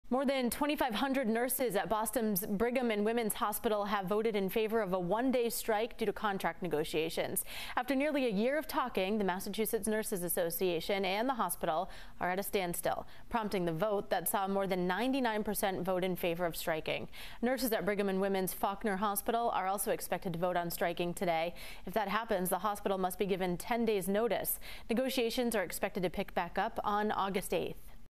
Thousands of nurses at Boston’s Brigham and Women’s Hospital voted in favor of one-day strike
More than 2,500 nurses at Boston’s Brigham and Women’s Hospital have voted in favor of a one-day strike due to contract negotiations After nearly a year of talking, the Massachusetts Nurses Association and the hospital are at a standstill, prompting the vote that saw more than 99% in favor of striking.